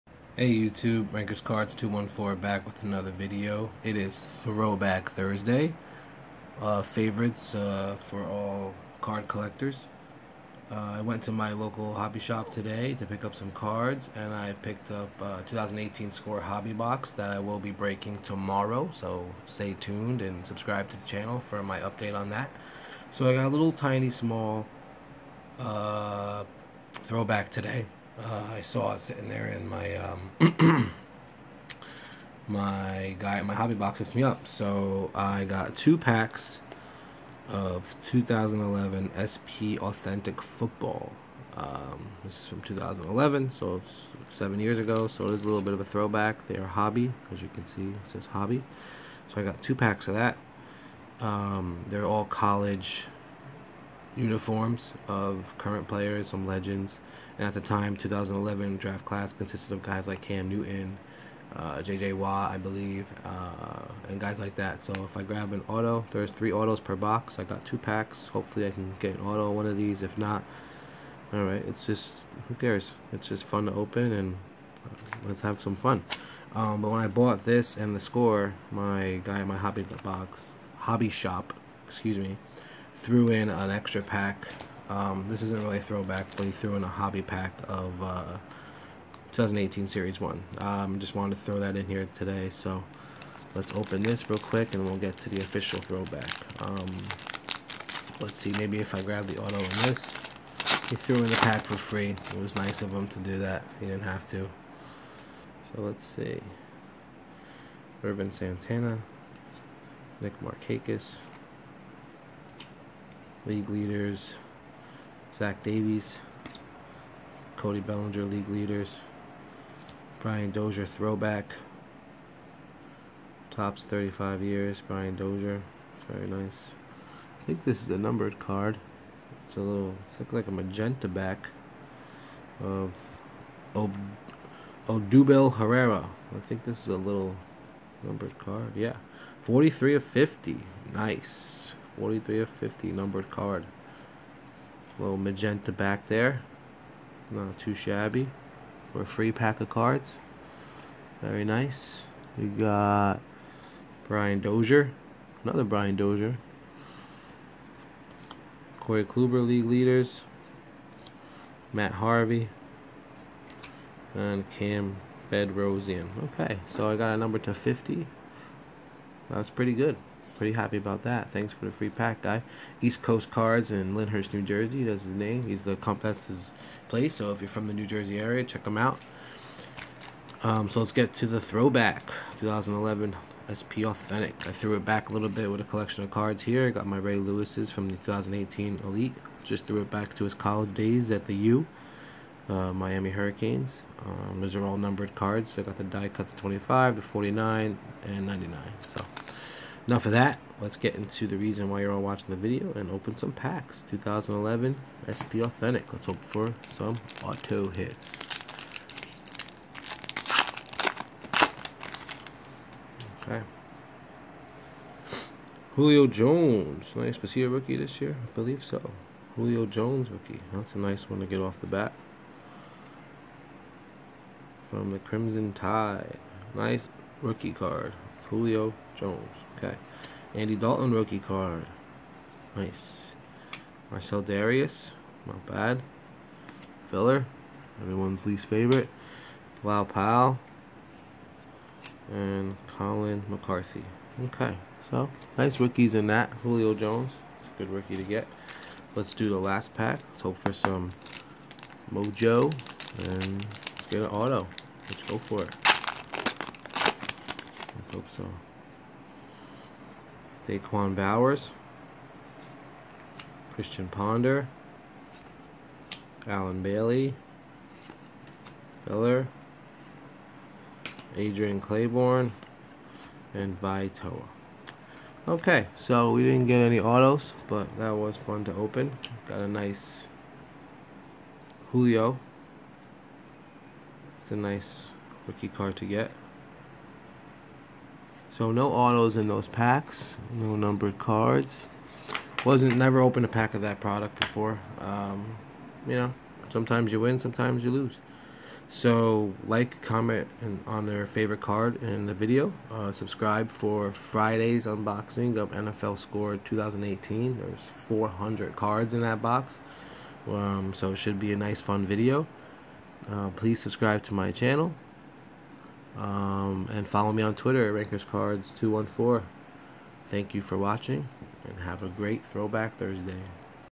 Throwback Thursday 2 Hobby Packs of 2011 SP Authentic Football Julio Rookie Card Free Hobby Pack?
A little Throwback Thursday action. 2 packs of 2011 SP Authentic Football. Great rookies! Free hobby pack of 2018 Topps series 1. Short print parallel!